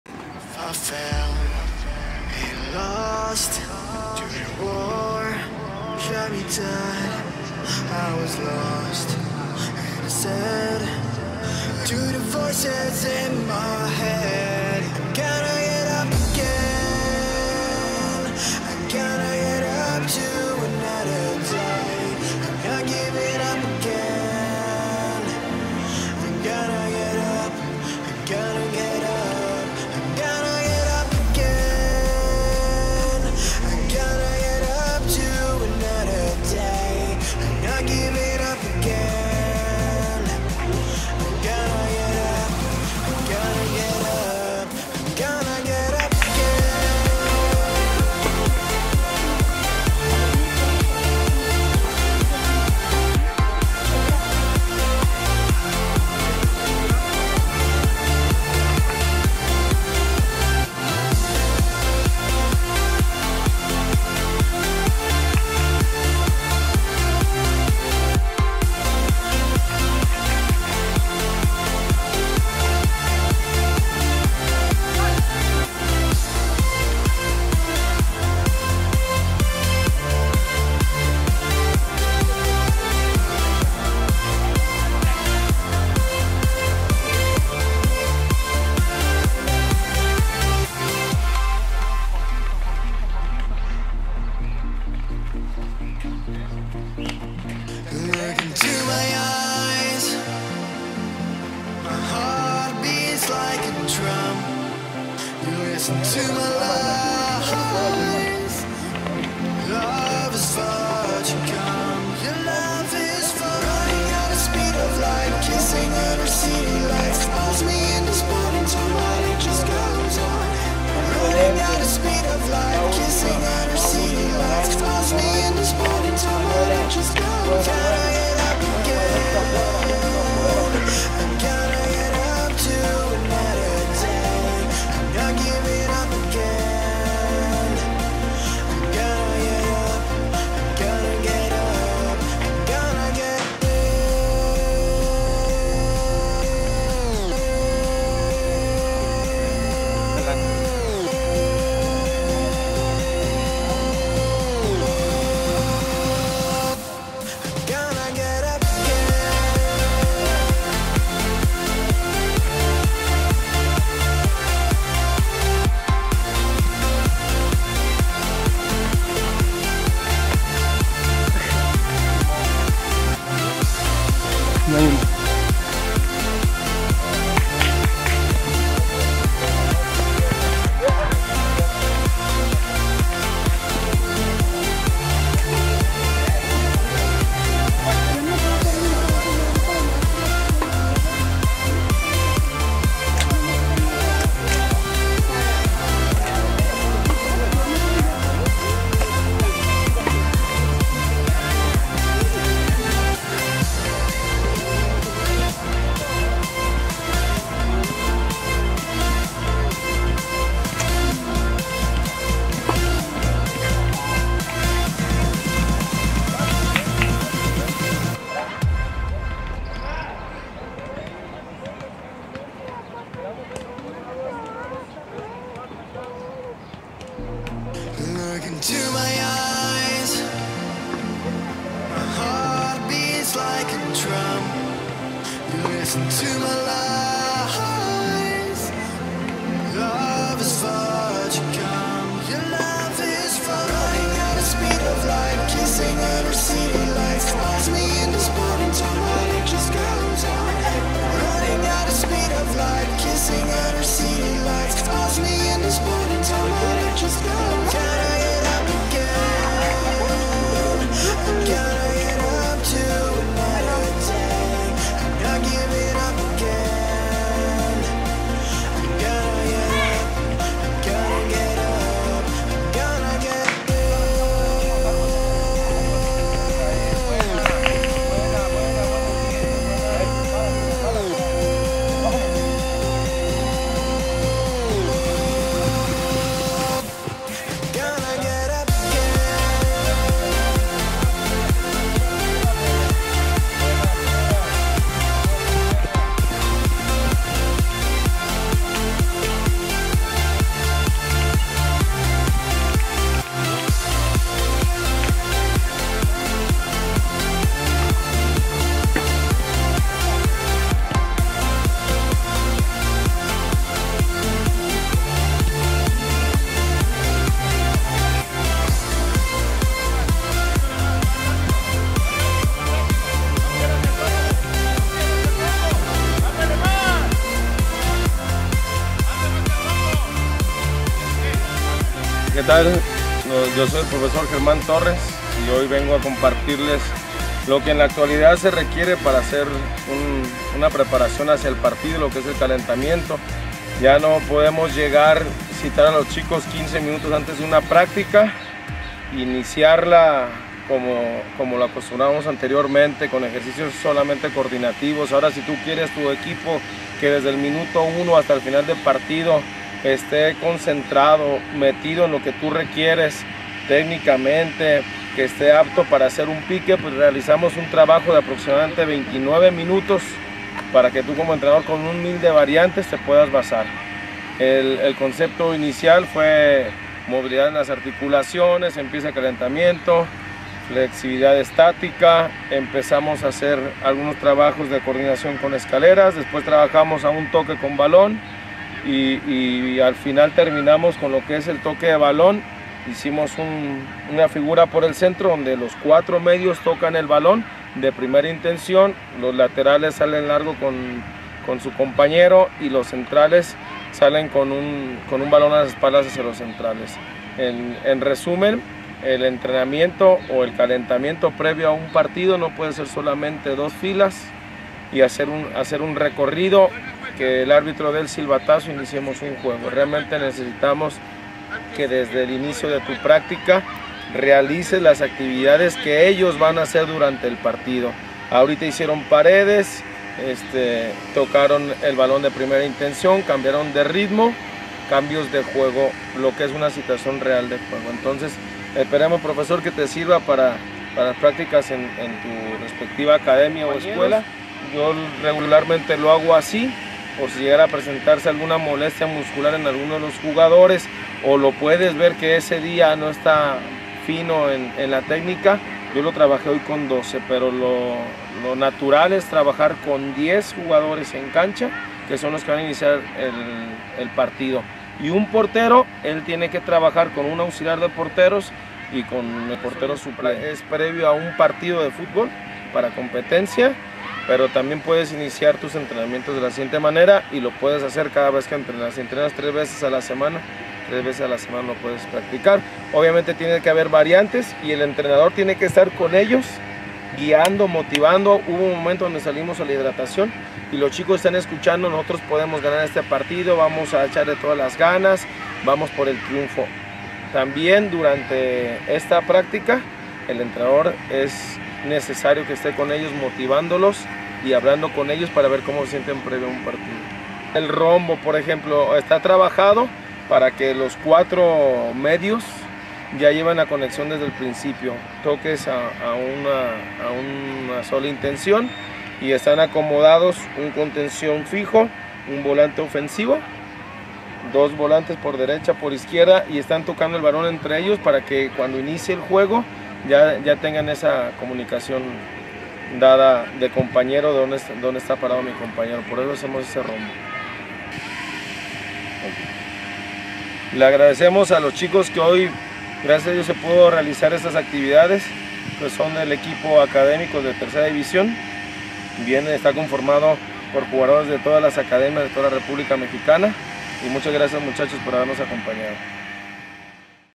Academias Atlas Preparación hacia el Partido
Tema impartido por el Prof. Germán Torres Velasco en el marco del Congreso de Entrenadores 2016